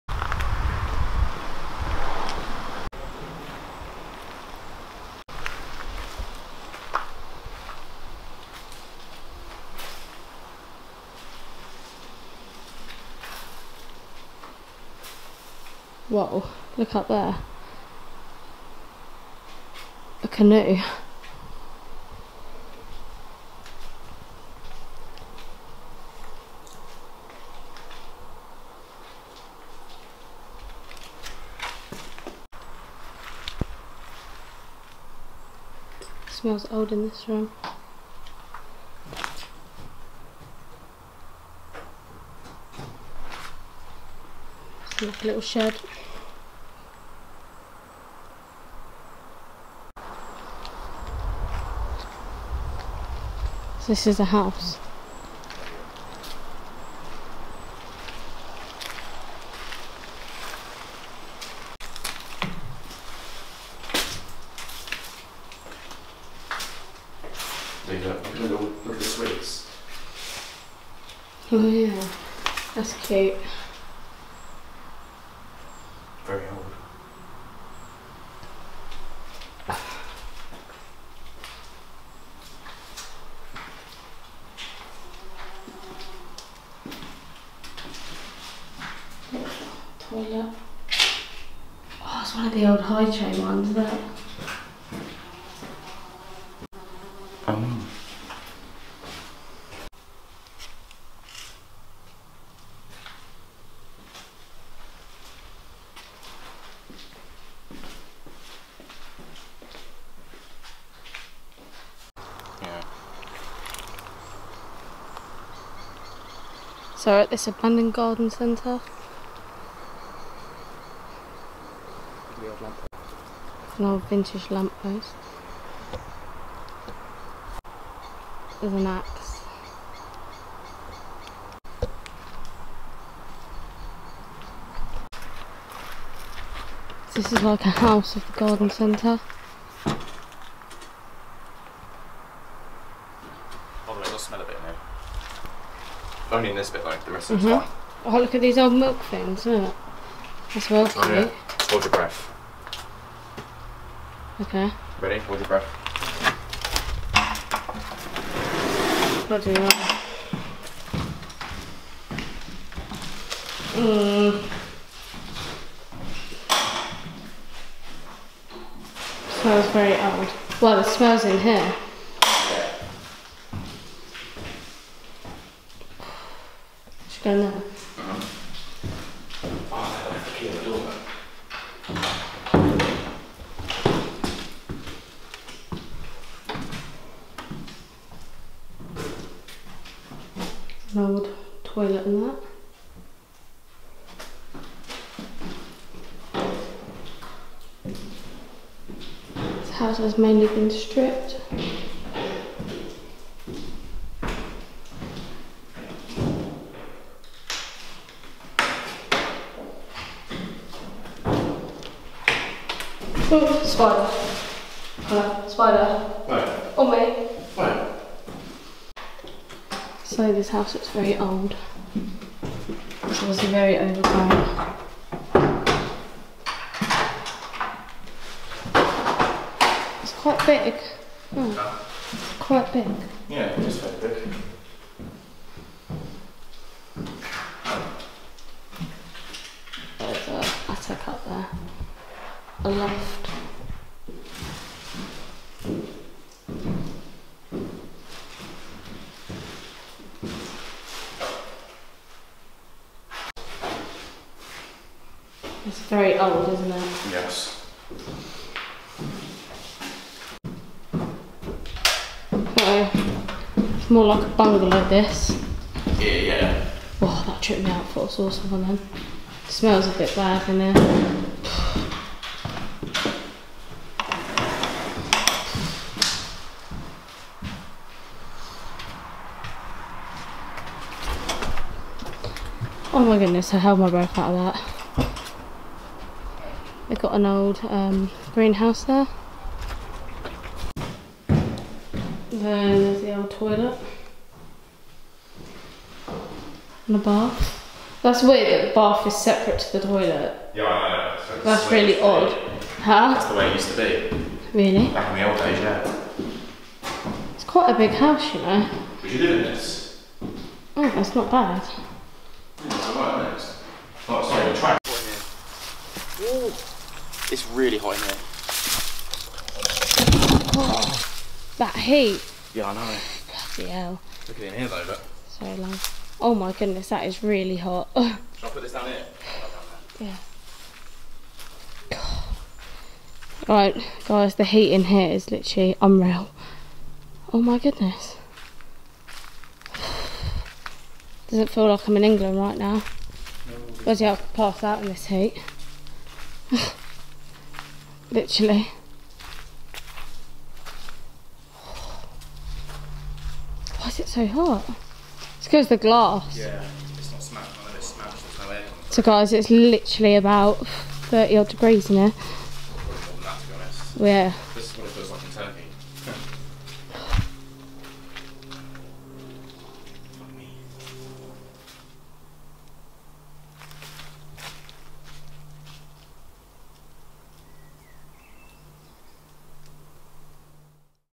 ABANDONED GARDEN CENTRE THINGS LEFT BEHIND
IN THIS VIDEO I VISITED AN ABANDONED GARDEN CENTRE!
►Thank you for all the support leave a nice comment bellow and enjoy the video!